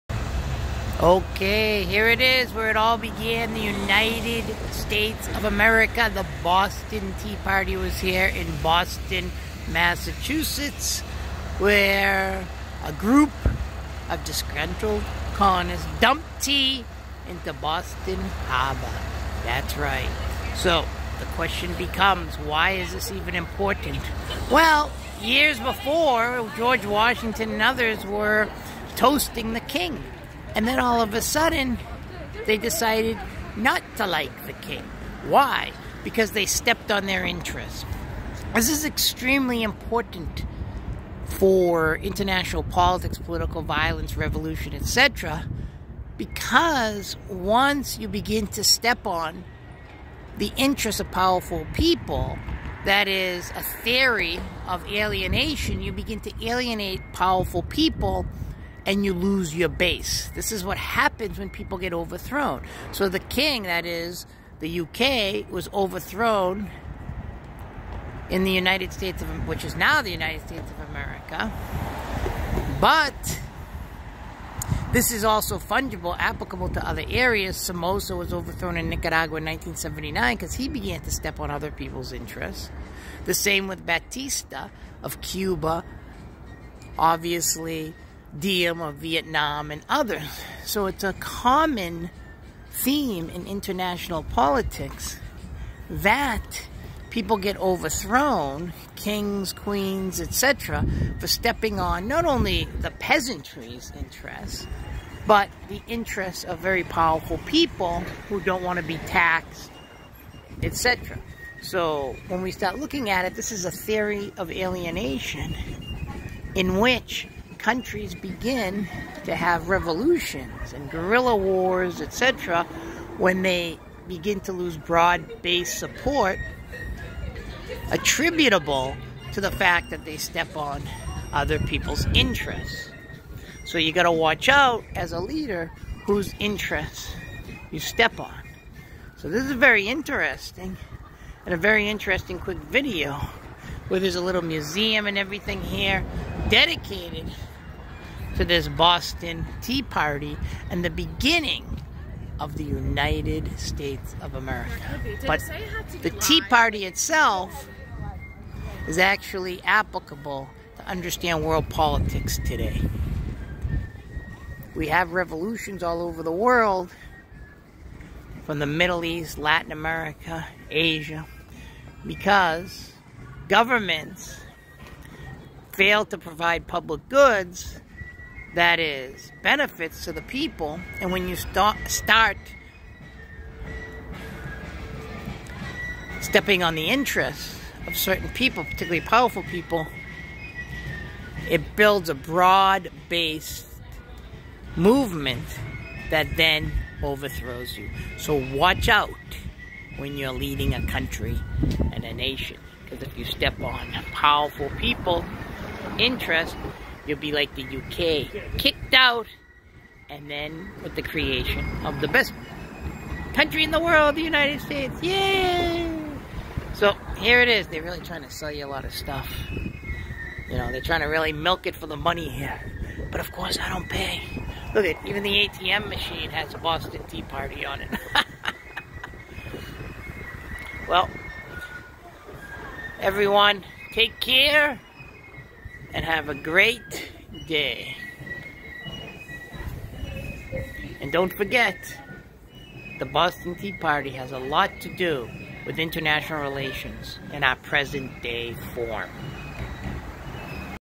Why Do Revolutions Occur? A Lesson from Boston!
This lecture takes place in Boston, MA--the home of the Boston Tea Party! It explains why the Revolutionary War occurred and how its origins can offer insight to historical, current, and future global unrest!